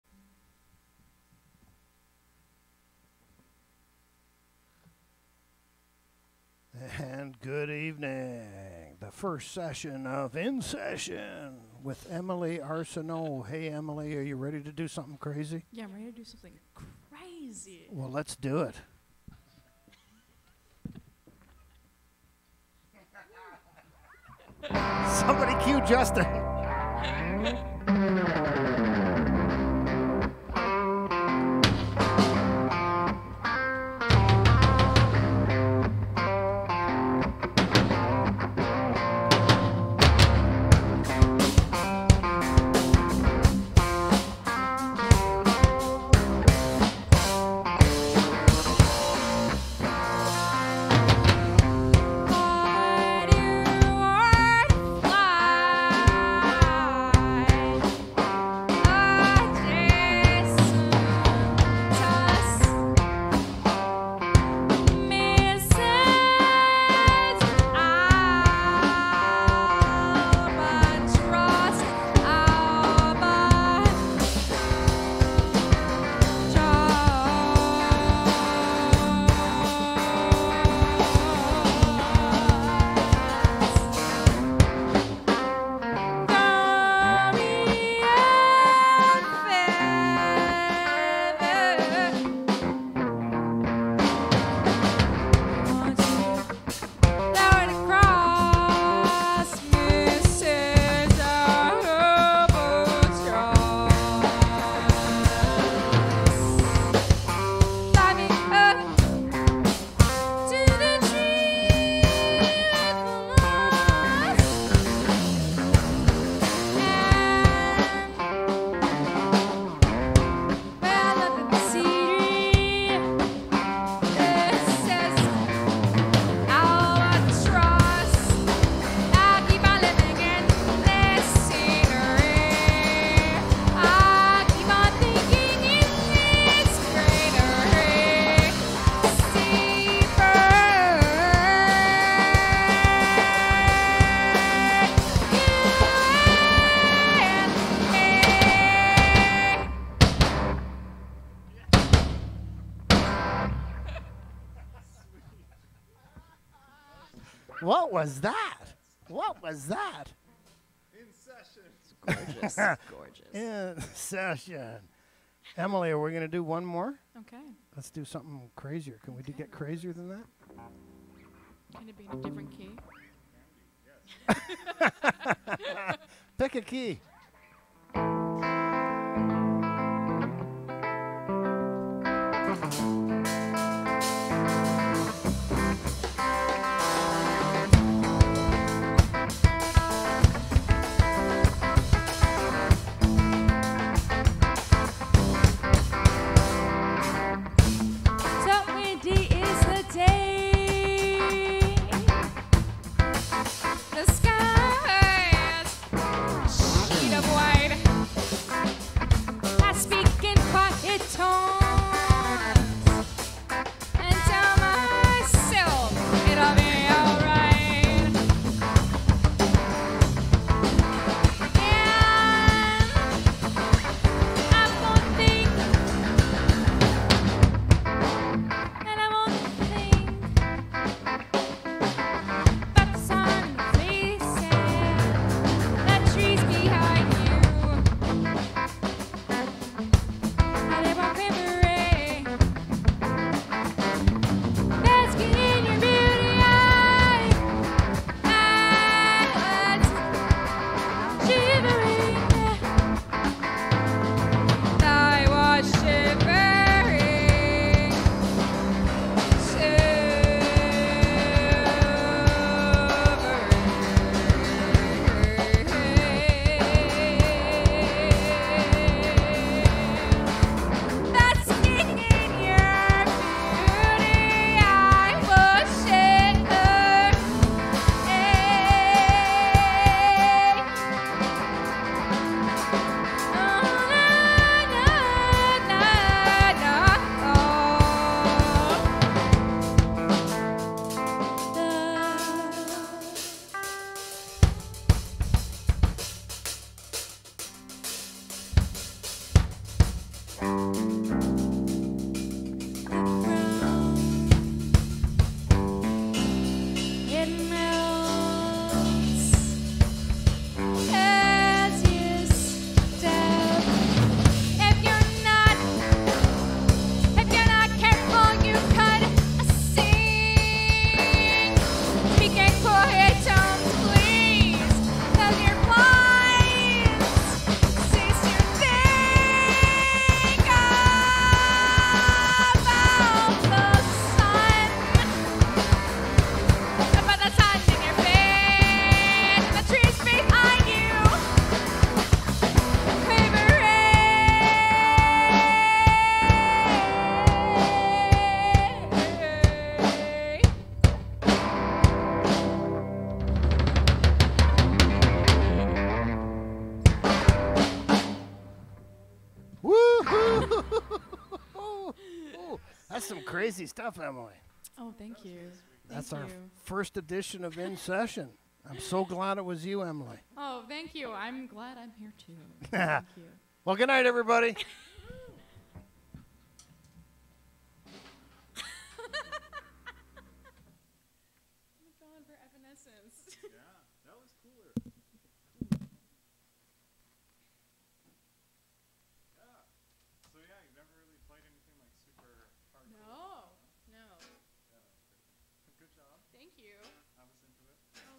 In Session with Emily Arsenault : a WinnipegStation Live Stream.
In Session with Emily Arsenault , with Scott Young on drums, and Justin Gaudry on guitar Live Stream Director Terry Birston